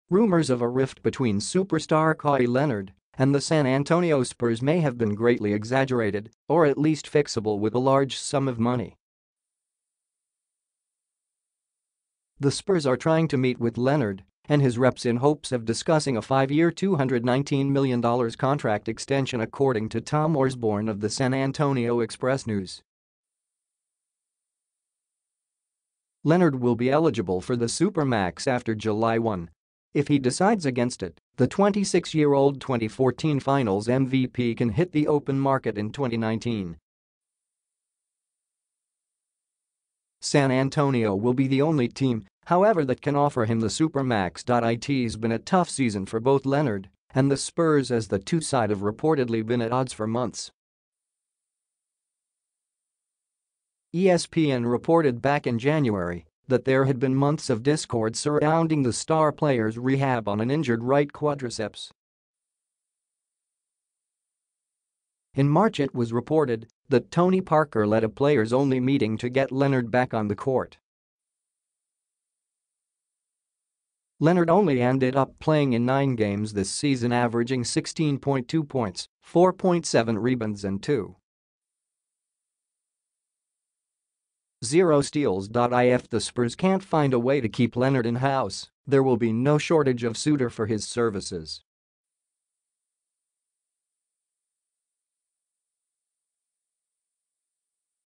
Spurs hoping to offer Kawhi Leonard supermax contract of $219 million
Rumours of a rift between superstar Kawhi Leonard and the San Antonio Spurs may have been greatly exaggerated, or at least fixable with a large sum of money.

The Spurs are trying to meet with Leonard and his reps in hopes of discussing a five-year $219-million contract extension according to Tom Orsborn of the San Antonio Express-News.

Leonard will be eligible for the super-max after July 1. If he decides against it, the 26-year-old 2014 Finals MVP can hit the open market in 2019. San Antonio will be the only team however that can offer him the super-max.

It's been a tough season for both Leonard and the Spurs as the two side have reportedly been at odds for months. ESPN reported back in January that there had been "months of discord" surrounding the star player's rehab on an injured right quadriceps.

In March it was reported that Tony Parker led a players-only meeting to get Leonard back on the court.

Leonard only ended up playing in nine games this season averaging 16.2 points, 4.7 rebounds and 2.0 steals.

If the Spurs can't find a way to keep Leonard in-house, there will be no shortage of suitor for his services.